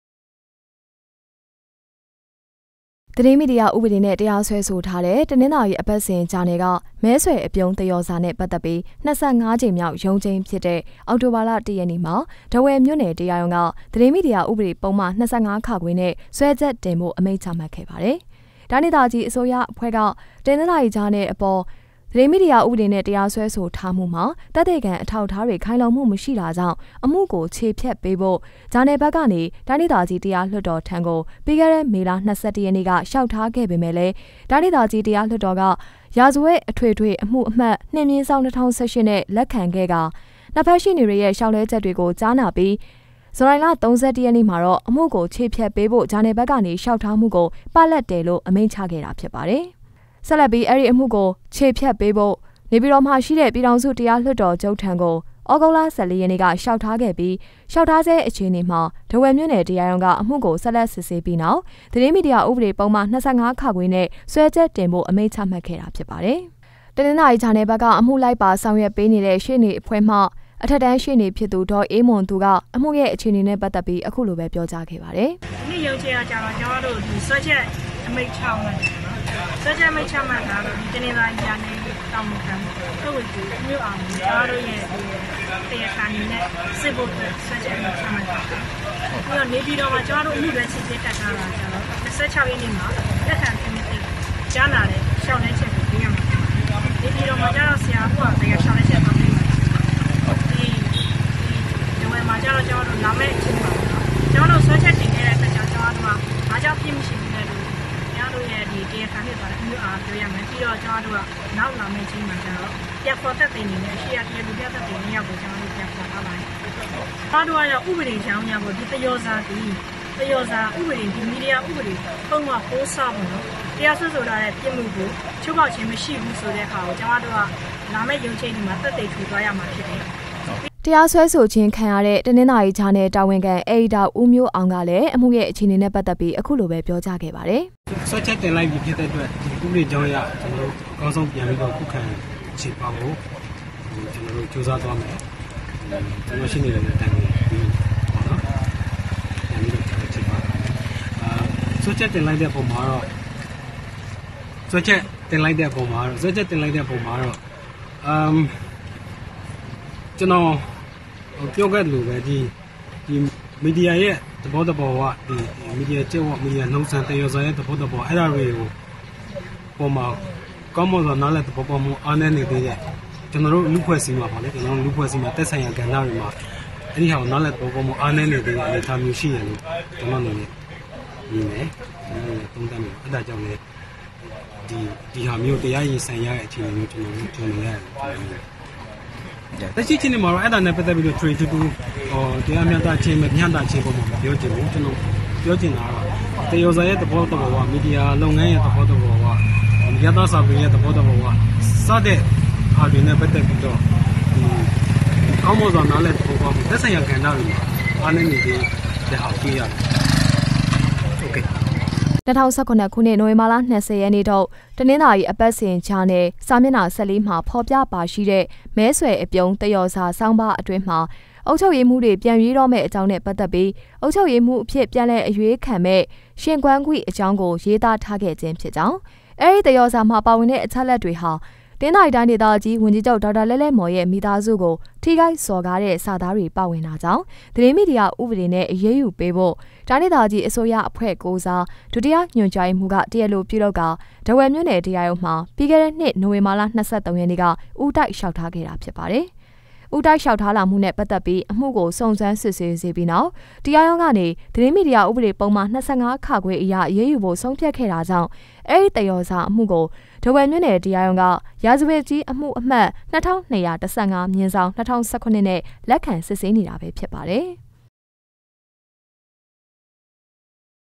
တနသာၤရီအပတ္စဥ္ဂ်ာနယ္၏ ‘မဲဆြယ္အျပံဳး’ သေရာ္စာအမႈႏွင့္ပတ္သက္၍
တနသာၤရီအပတ္စဥ္ဂ်ာနယ္၏ ‘မဲဆြယ္အျပံဳး’ သေရာ္စာအမႈႏွင့္ပတ္သက္၍ သတင္းမီဒီယာဥပေဒပုဒ္မ ၂၅ (ခ) ျဖင့္ စဲြခ်က္တင္ရန္ အမိန္႔ခ်မွတ္ခ့ဲ

သတင္းမီဒီယာဥပေဒျဖင့္ တရားစြဲဆိုထာတဲ့ တနသာၤရီအပတ္စဥ္ဂ်ာနယ္က ‘မဲဆြယ္အျပံဳး’ သေရာ္စာနဲ႔ပတ္သက္ၿပီး ၂၅ ႀကိမ္ေျမာက္ရံုးခ်ိန္းျဖစ္ေသာ ေအာက္တိုဘာလ ၁ ရက္ေန႔တြင္ ထားဝယ္ၿမိဳ႕နယ္တရားရံုးက သတင္းမီဒီယာဥပေဒပုဒ္မ ၂၅ (ခ) ျဖင့္ စဲြခ်က္တင္ရန္ အမိန္႔ခ်မွတ္ခ့ဲပါတယ္။